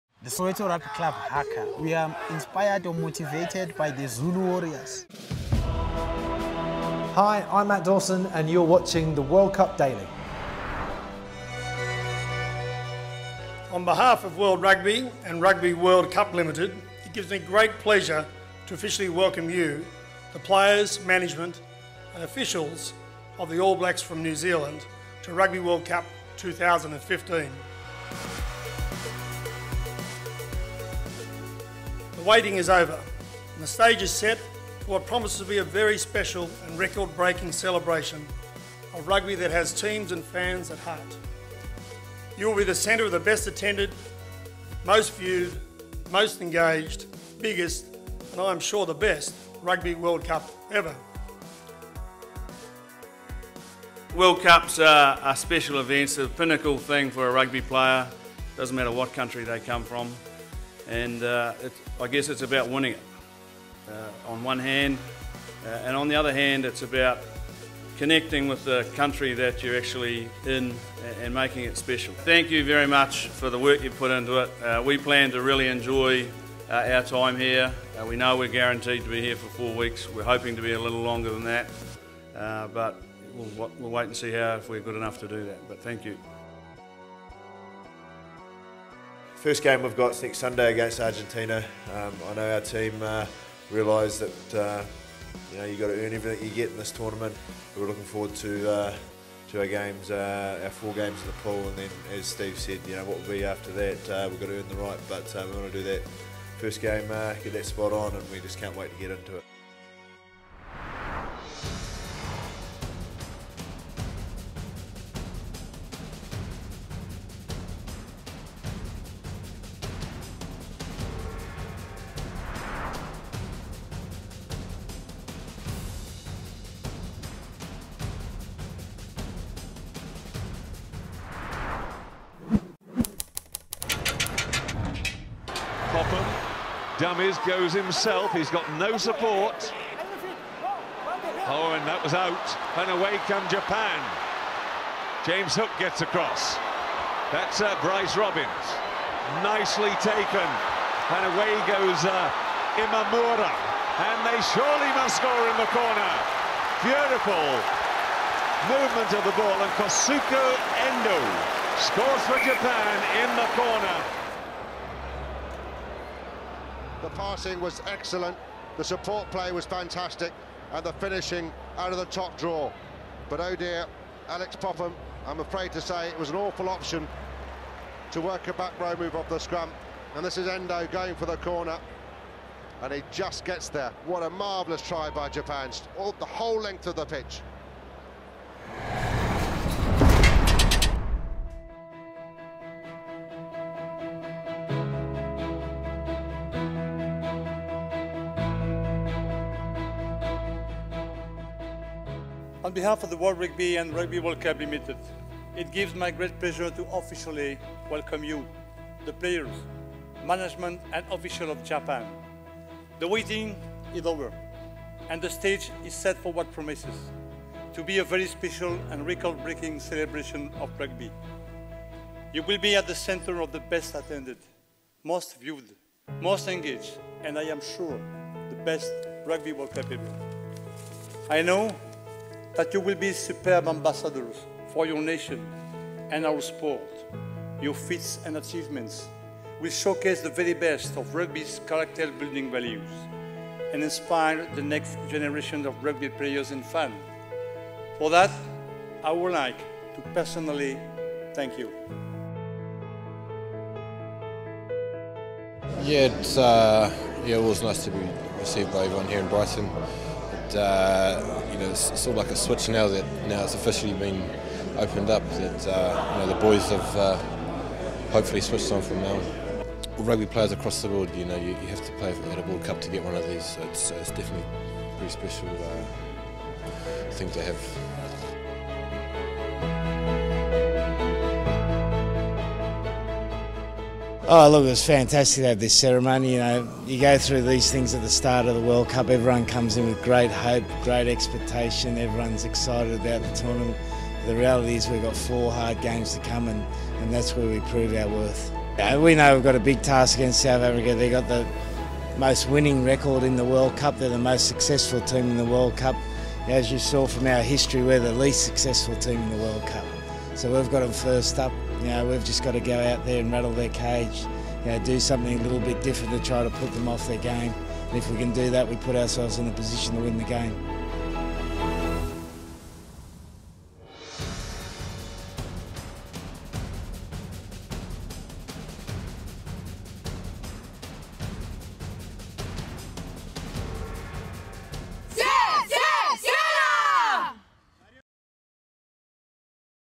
McCaw: 'All Black's first game has to be spot on' - RWC Daily
In the 2nd episode of the Land Rover Rugby World Cup daily, we see the New Zealand and Japan teams arrive in England, and take a look back of one of the great tries in recent RWC history from Japan's Endo.

TWITTER.COM/RUGBYWORLDCUP
FACEBOOK.COM/RUGBYWORLDCUP
FACEBOOK.COM/WORLDUGBY
INSTAGRAM.COM/WORLDRUGBY